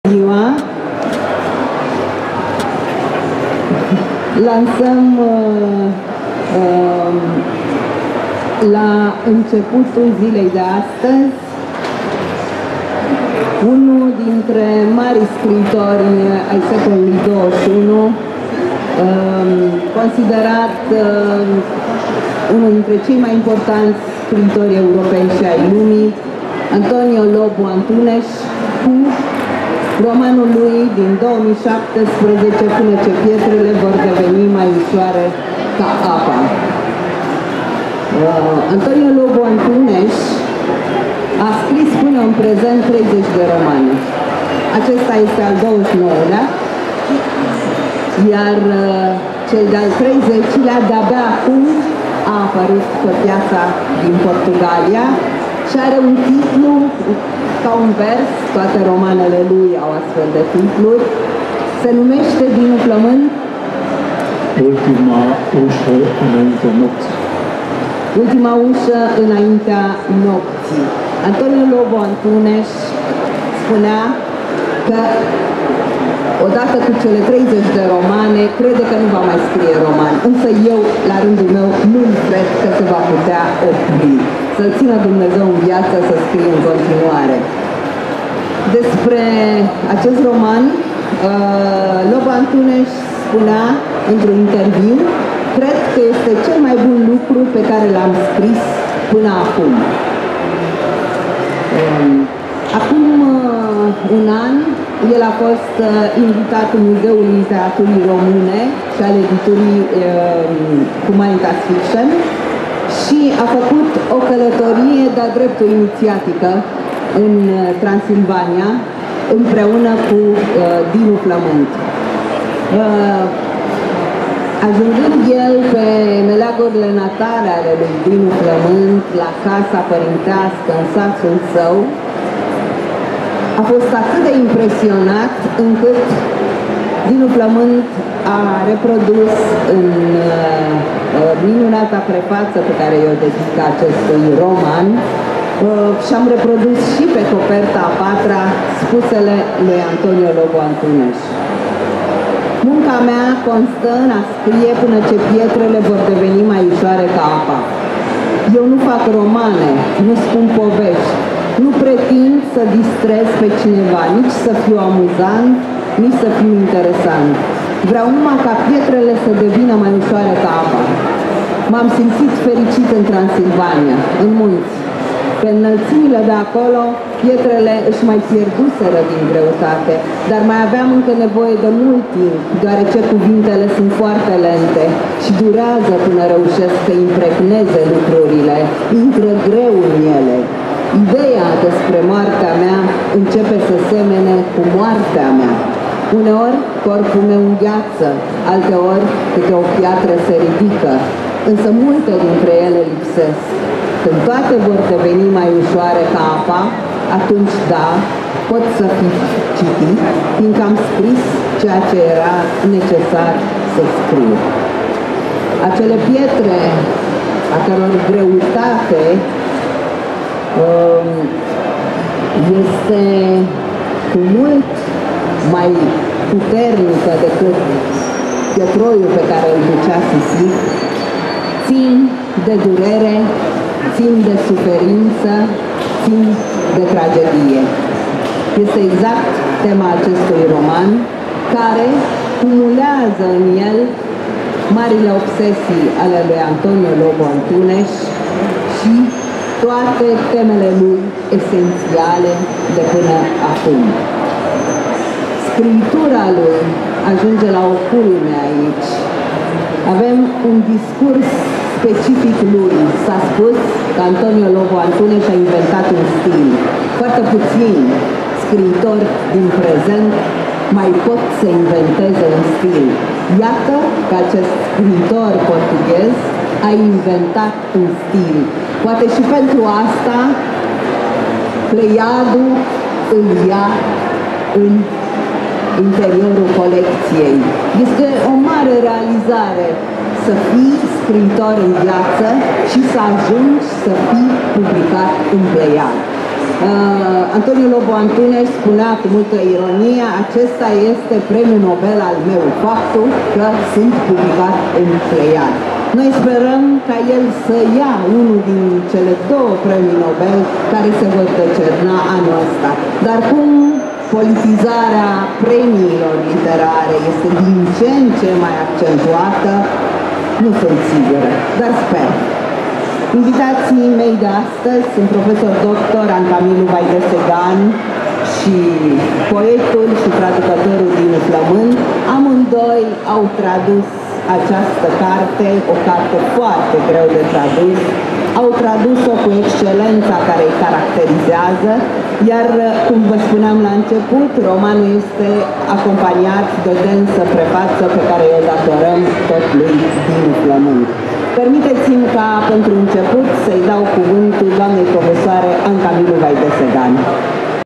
(01) Denisa Comănescu: Lansare "Pănă ce pietrele vor deveni mai ușoare ca apa", Antonio Lobo Antunes
În data de 2 iunie 2019 a avut loc lansarea de carte "Pănă ce pietrele vor deveni mai ușoare ca apa" de Antonio Lobo Antunes, colecția "Raftul Denisei", Humanitas Fiction, în cadrul târgului Bookfest 2019.

Vorbitori în cadrul evenimentului: Dinu Flămând, Anca Milu Vaidesegan, Denisa Comănescu.